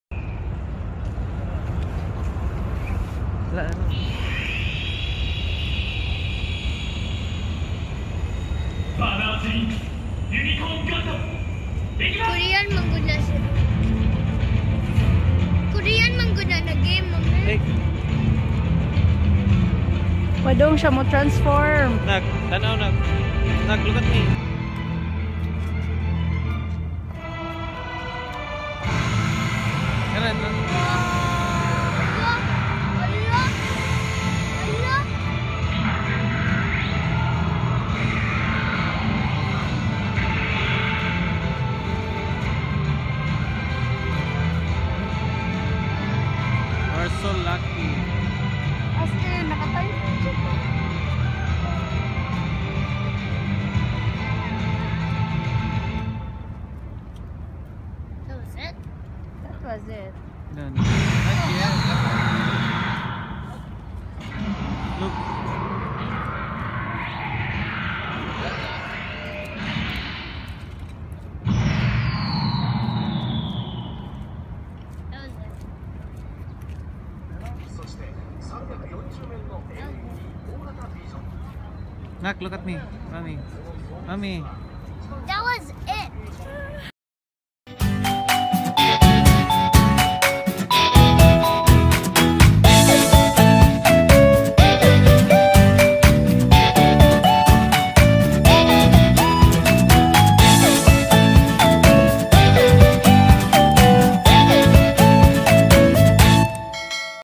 WATCH! The world's largest Unicorn Gundam, TRANSFORMED! | Gundam Base Tokyo
Published on sept. 10, 2020.

Giant Gundam Unicorn statue is found in Odaiba, Japan. It is modeled after a RX-78-2 Gundam. It transform from Unicorn Mode to Destroy Mode at a certain schedule.